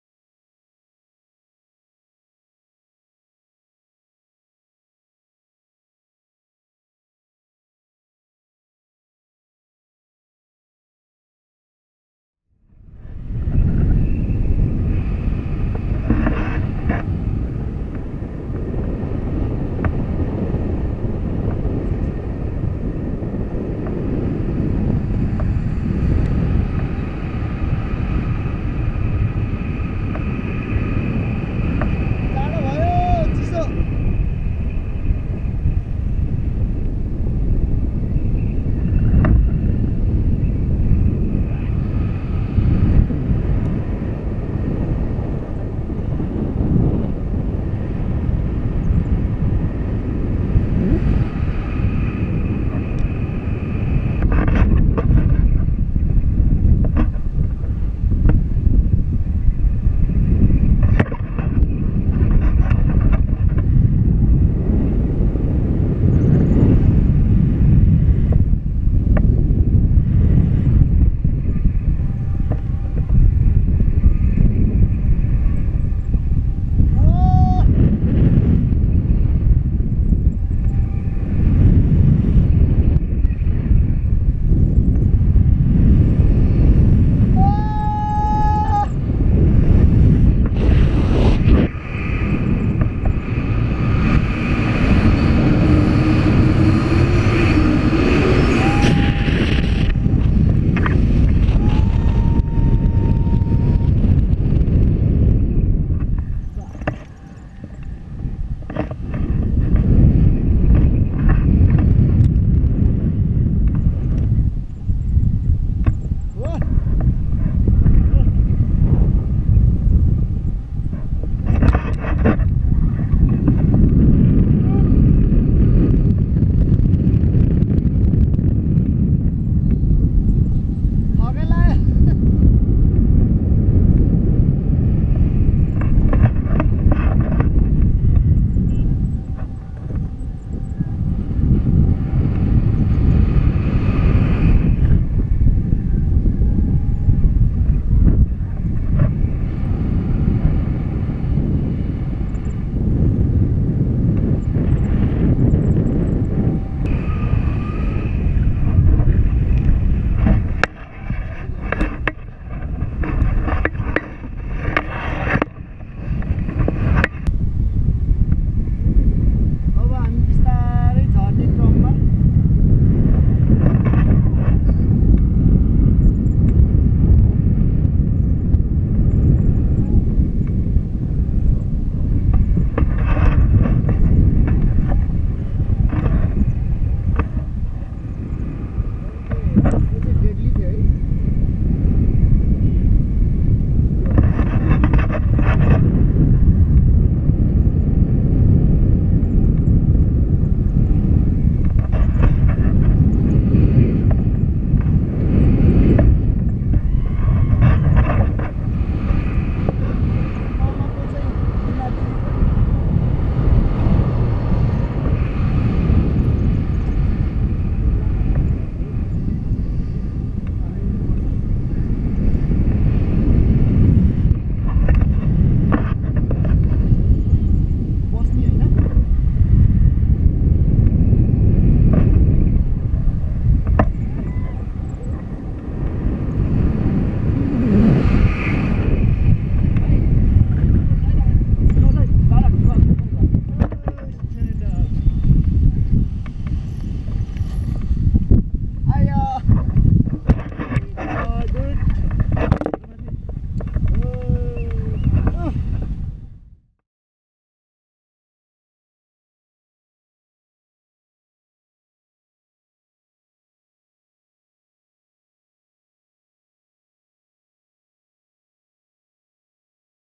PARAGLIDING FROM SARANGKOT | AEROLIGHT PARAGLIDING, LAKESIDE POKHARA | PILOT :- SUBIN RANA
Paragliding from sarangkot  big thanks to Aerolight paragliding and pilot Subin Rana. It was amazing experience, it was my first time. I personally suggest you all to visit AEROLIGHT PARAGLIDING which is located in Lakeside Pokhara. I guarantee, you will have amazing time. thank you.